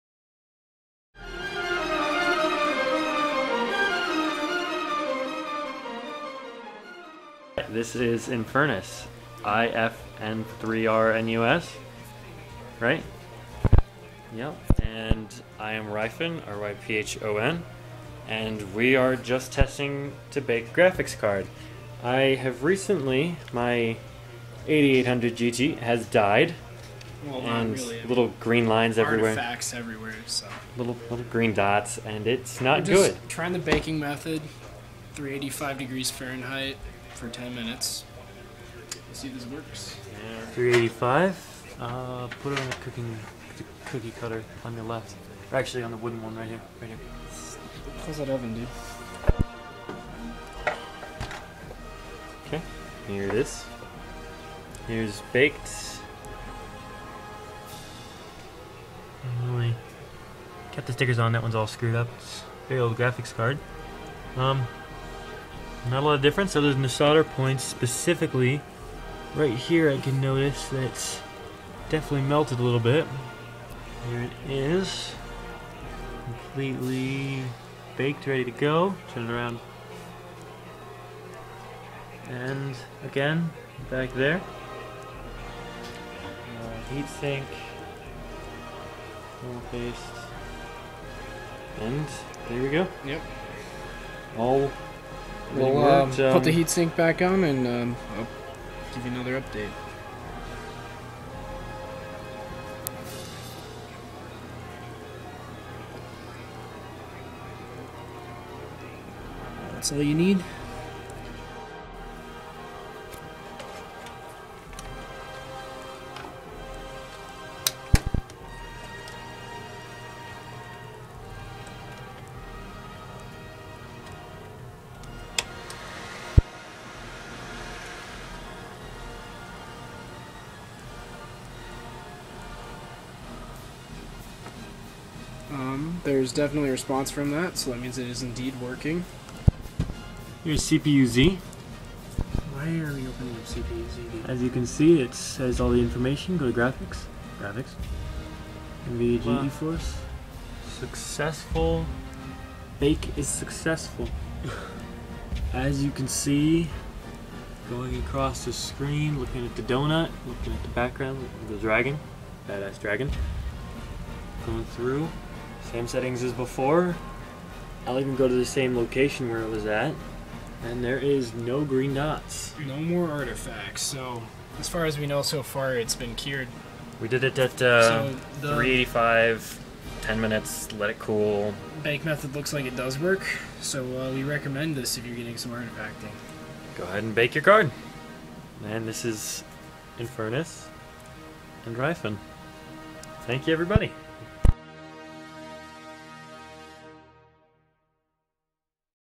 8800GT Baking
So... Baked the card at 385 for 10 minutes. Everything worked out pretty well. Went from artifacts to this.